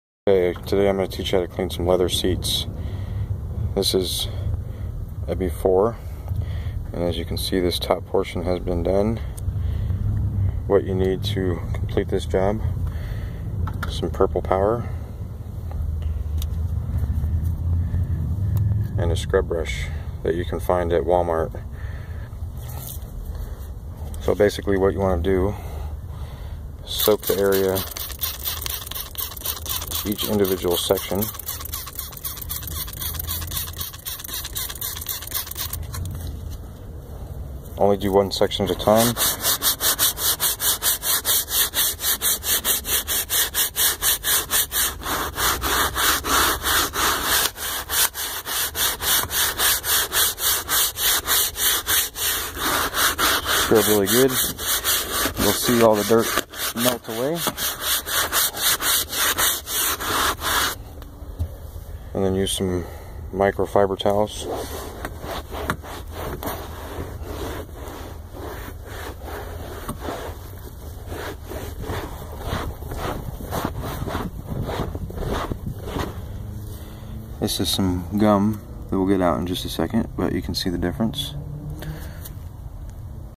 How to clean real dirty leather seats in a car video
How to clean dirty leather seats in your car, truck, boat etc. This technique can be used on any leather. Cleaning leather on a car in two easy steps